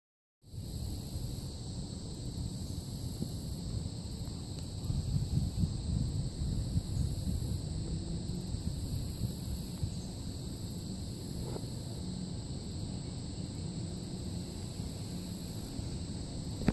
Butterflies Drinking Shrimp Juice
Have you ever seen butterflies eating shrimp?